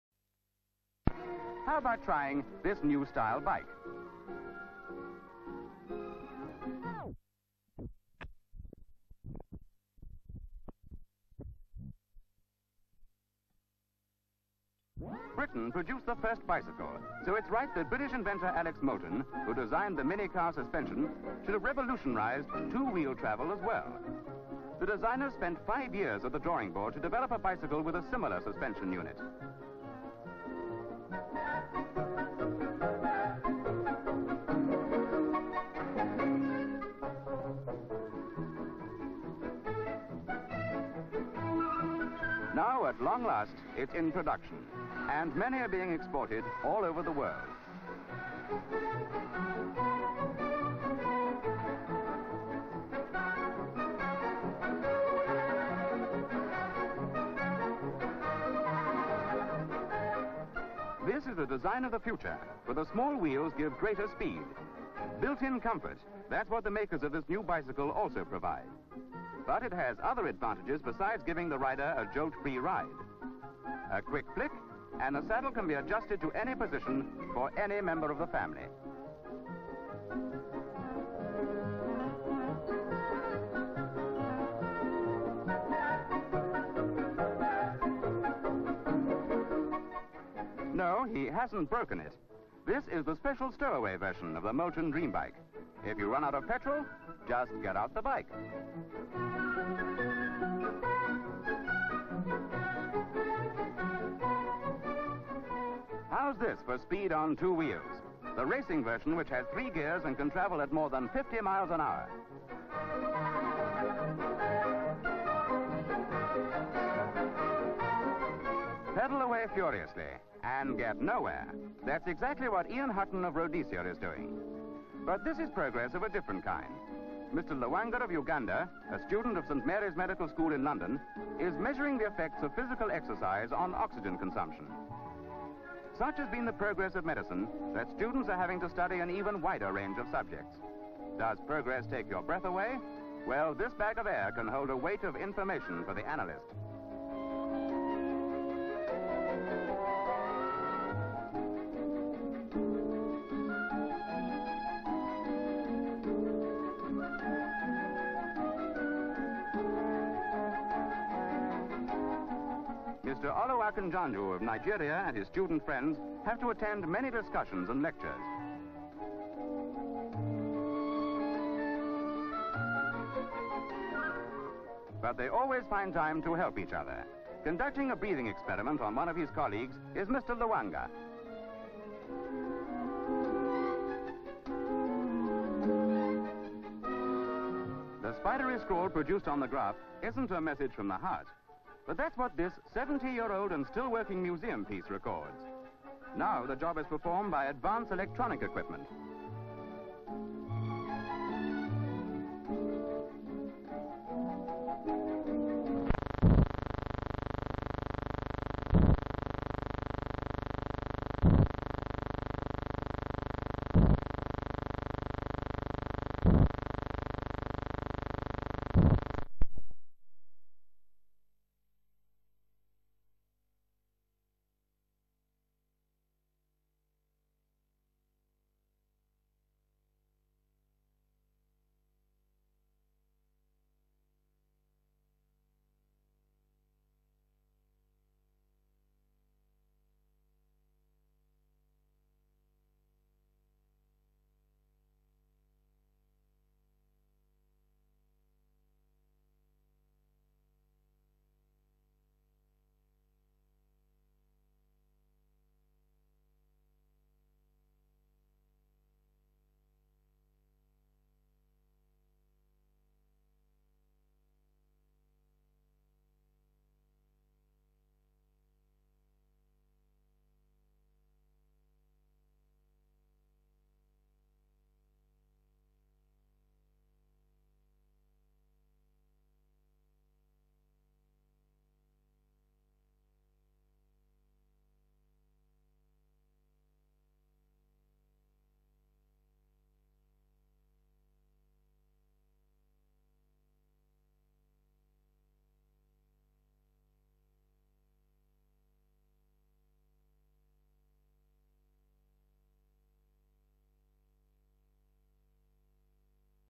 Cycle Race At Olympia, 1960's - Film 4286
Documentary about bicycle design.  Models of bikes.  Man cycles along road.  British inventor Alex Molton (designer of mini car suspension) with another man bent over designer's drawing board.  Life size drawing of bicycle.  Man holds metal parts of bike over the drawings.  Tests the suspension of the front fork.  Demonstrates couplings.  Welding.  Man with oxy-acetylene torch and wearing protective goggles.  Engineers build bike.  Testing brakes, fitting chain, tightening bolts.  Bike has two small wheels.  Adjustable saddle.  Woman fits shopping bag on back of bike before riding off.  Man pulls bike into two pieces and stores them in the back of his car, a Morris 1100.  Molton rides down side road and sports cyclist emerges from same road on same type of bicycle with small wheels.  It has sprinters drop handlebars.  Chocked exercise bike in laboratory with man.  Man is pedalling fast and wears respirator.  Doctors (one black) monitor man.  Exhaled air is examined.  Scientist in classroom lectures.  Uses board duster.  Breathing experiment, graph draws lines.  70 year old heart monitor machine (which runs on little railway track).  Modern medical monitoring equipment. [Ends here].

Olympia.  Six day cycle race (b/w-silent).  Cyclist rides round track with one foot on handlebars whilst eating.  Fast racing.  Overtaking slower riders.  Very, very tight banking on tight bends.  Good racing.  Velodrome.